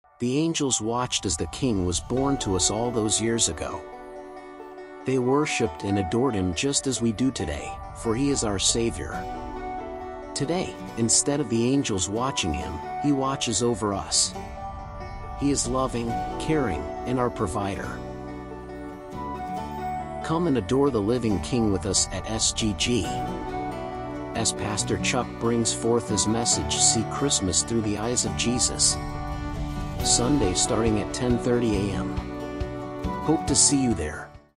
Join us at SGG this Sunday
join us at sgg or live stream on YouTube and Facebook for Pastor Chuck sermon see Christmas Through The Eyes of Jesus this Sunday starting at 10:30 a.m. 480 East Market Street Warren Ohio 4445 hope to see you there